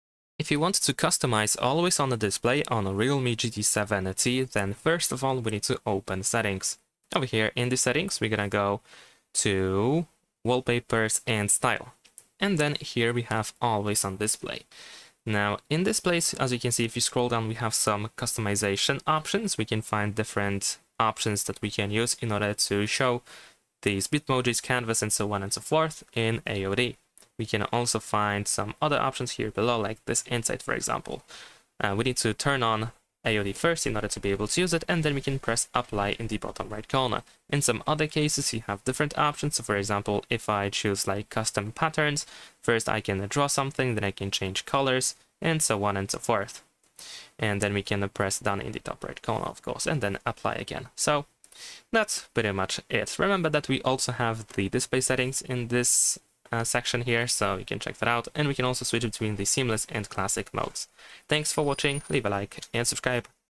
Realme GT 7T: How to Customize Always On Display?
Learn step-by-step how to personalize your Always On Display settings for clocks, notifications, and styles. We explore all AOD customization options including themes, schedules, and battery-saving tips for your Realme GT 7T. Watch this tutorial to master every Always On Display feature on your device.